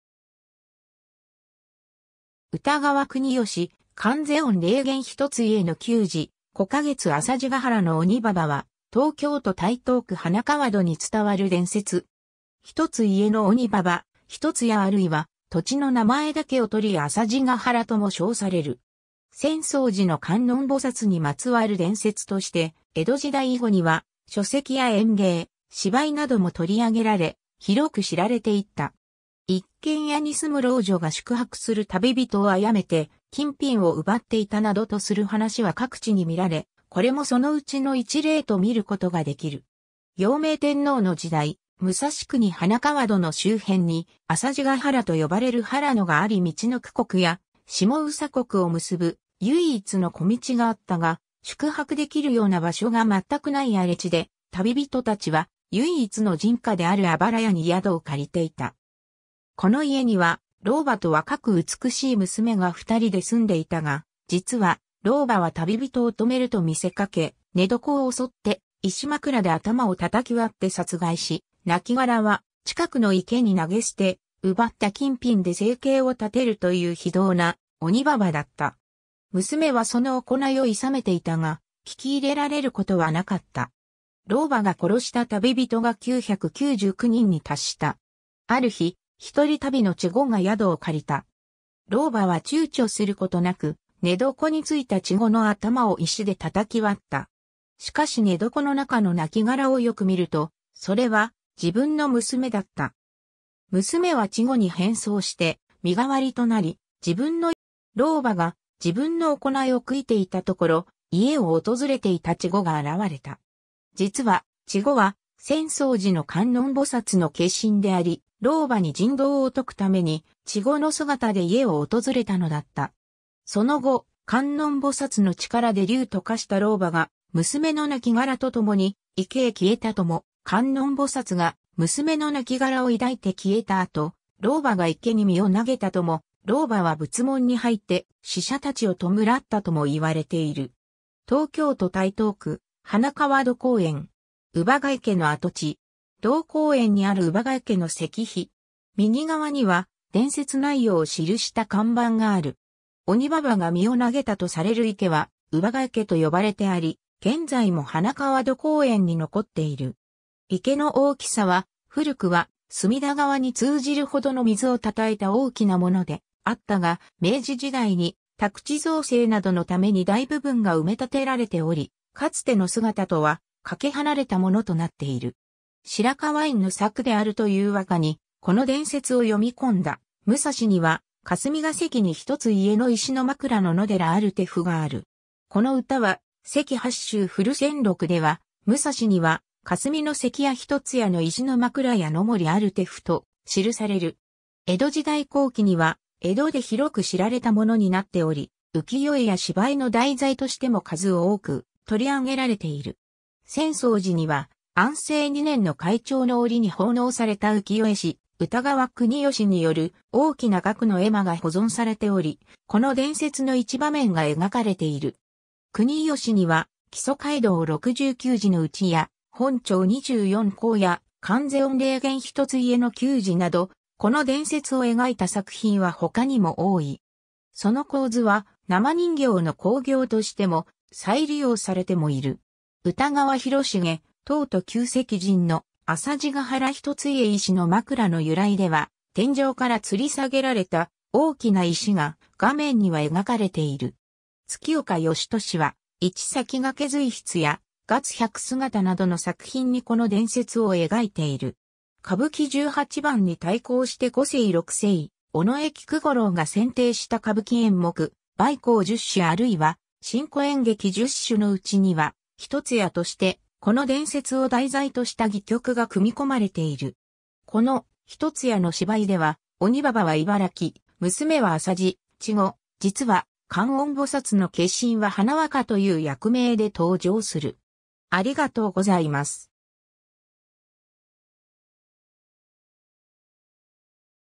浅茅ヶ原の鬼婆
歌川国芳『観世音霊験一ツ家の旧事』 「孤家月」（ひとつやのつき）（月岡芳年『月百姿』のうちの一作） 浅茅ヶ原の鬼婆（あさぢがはらのおにばば）は、東京都台東区花川戸に伝わる伝説。
一つ家の鬼婆（ひとつやのおにばば）、一つ家（ひとつや）あるいは土地の名前だけをとり浅茅ヶ原（あさぢがはら）とも称される。
 浅草寺（東京都台東区）の観音菩薩にまつわる伝説として江戸時代以後には書籍や演芸・芝居なども取り上げられ、広く知られていった。
一軒家に棲む老女が宿泊する旅人をあやめて金品を奪っていたなどとする話は各地にみられ、これもそのうちの一例と見ることができる。
 用明天皇の時代、武蔵国花川戸の周辺に浅茅ヶ原と呼ばれる原野があり陸奥国や下総国を結ぶ唯一の小道があったが、宿泊できるような場所がまったくない荒地で、旅人たちは唯一の人家であるあばら家に宿を借りていた。
この家には老婆と若く美しい娘が2人で住んでいたが、実は老婆は旅人を泊めると見せかけ、寝床を襲って石枕で頭を叩き割って殺害し（『関八州古戦録』巻二では、天井から縄をつけた大石を落として圧殺したと記す）、亡骸は近くの池に投げ捨て、奪った金品で生計を立てるという非道な鬼婆だった。
娘はその行いを諌めていたが、聞き入れられることはなかった。
老婆が殺した旅人が999人に達した。
ある日、ひとり旅の稚児（ちご）が宿を借りた。
老婆は躊躇することなく、寝床についた稚児の頭を石で叩き割った。
しかし寝床の中の亡骸をよく見ると、それは自分の娘だった。
娘は稚児に変装して身代わりとなり、自分の命をもって老婆の行いを咎めようとしていたのだった。
老婆が自分の行いを悔いていたところ、家を訪れていた稚児が現れた。
実は稚児は浅草寺の観音菩薩の化身であり、老婆に人道を説くために稚児の姿で家を訪れたのだった。
その後、観音菩薩の力で竜と化した老婆が娘の亡骸とともに池へ消えたとも、観音菩薩が娘の亡骸を抱いて消えた後、老婆が池に身を投げたとも、老婆は仏門に入って死者たちを弔ったともいわれている。
 東京都台東区・花川戸公園。
姥ヶ池の跡地。
 同公園にある姥ヶ池の石碑。
右側には伝説内容を記した看板がある。
 鬼婆が身を投げたとされる池は姥ヶ池（うばがいけ）と呼ばれてあり、現在も花川戸公園に残っている。
池の大きさは、古くは隅田川に通じるほどの水をたたえた大きなものであったが、明治時代に宅地造成などのために大部分が埋め立てられており、かつての姿とはかけ離れたものとなっている。
 白河院の作であるという和歌に、この伝説を詠み込んだ「武蔵には霞が関に一つ家の石の枕の野寺あるてふ」がある。
この歌は、『関八州古戦録』では、「武蔵には霞の関屋ひとつ屋の石のまくらや野守あるてふ」と記される。
 江戸時代後期には江戸で広く知られたものになっており、浮世絵や芝居の題材としても数多く取り上げられている。
浅草寺（東京都台東区）には安政2年（1855年）の開帳のおりに奉納された浮世絵師・歌川国芳による大きな額の絵馬が保存されており、この伝説の一場面（諌める娘を恫喝する鬼婆）が描かれている。
国芳には『木曽街道六十九次之内』（「大久手 一ツ家老婆」）や『本朝二十四孝』（「一ツ家の老女」）や『観世音霊験一ツ家の旧事』など、この伝説を描いた作品は他にも多い。
その構図は生人形の興行としても再利用されてもいる。
 歌川広重『東都旧跡尽』の「浅茅ヶ原一ツ家 石の枕の由来」では、天井からつり下げられた大きな石が画面には描かれている。
月岡芳年は、『一魁随筆』（「一ッ家老婆」）や『月百姿』（「孤家月」）などの作品にこの伝説を描いている。
 歌舞伎十八番に対抗して5世・6世尾上菊五郎が選定した歌舞伎演目「梅幸十種」あるいは「新古演劇十種」のうちには「一つ家」として、この伝説を題材とした戯曲が組み込まれている。
この「一つ家」の芝居では、鬼婆は茨木（いばらき）、娘は浅茅（あさじ）、稚児・実は漢音菩薩の化身は花若（はなわか）という役名で登場する。